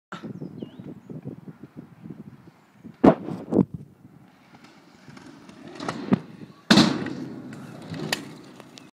Marlwood scooter skills
Will doin a 180 transfer on a jdbug